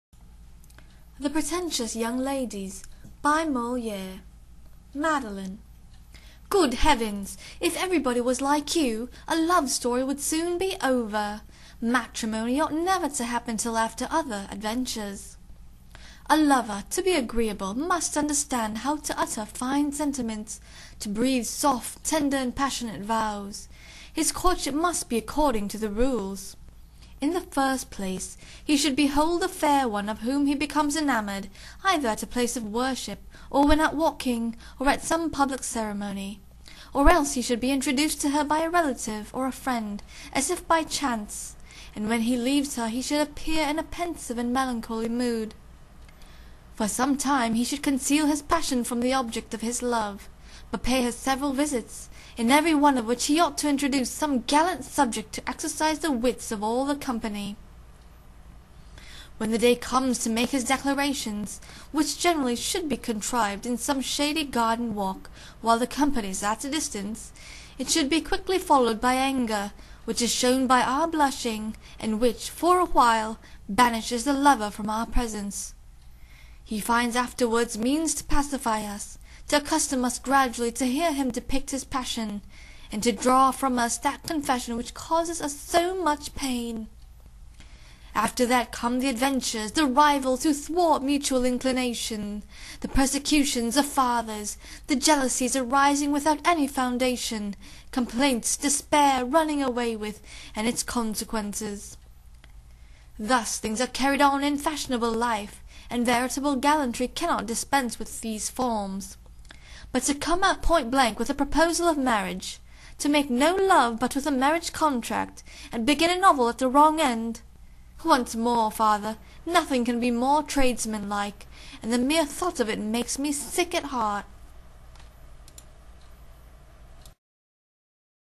The Pretentious Young Ladies by Moliere
Monologue of Madelon from The Pretentious Young Ladies by Moliere